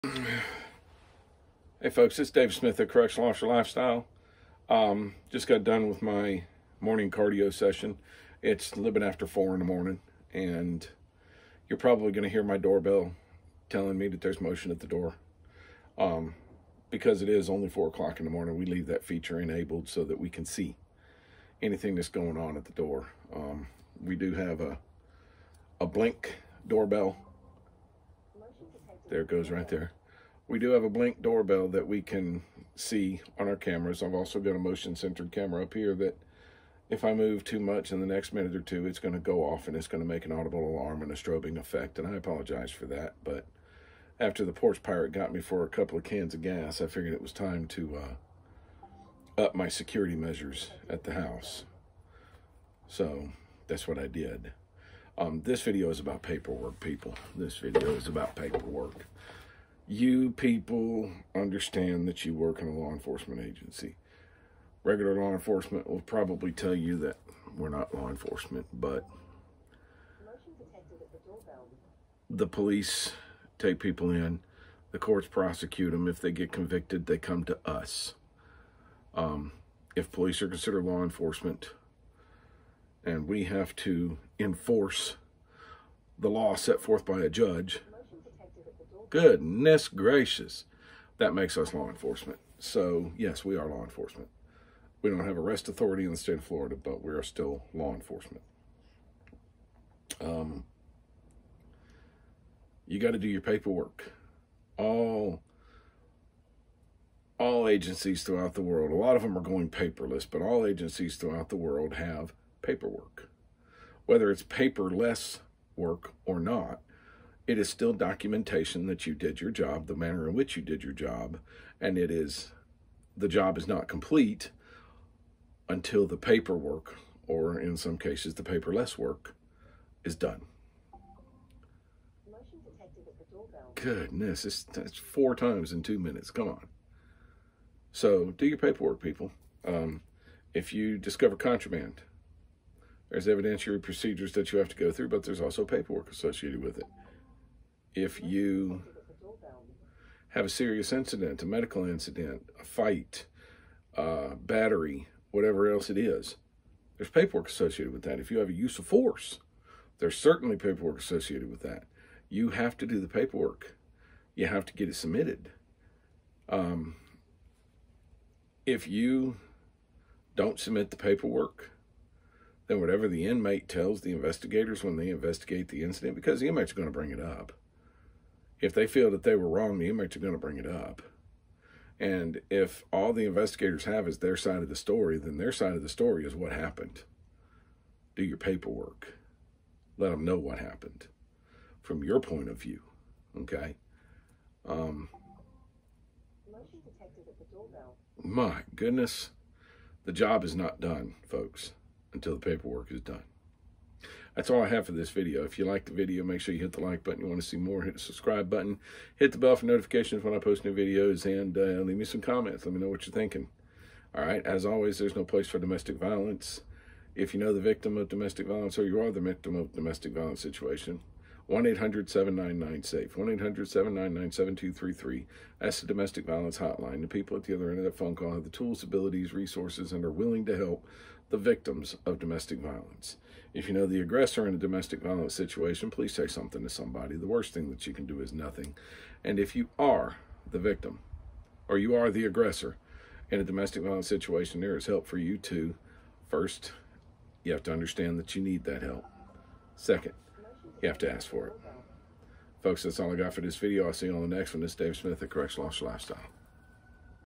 Paperwork
This is a video about getting your paperwork done.  I am going to go ahead and apologize for the doorbell going haywire.  Please enjoy the video.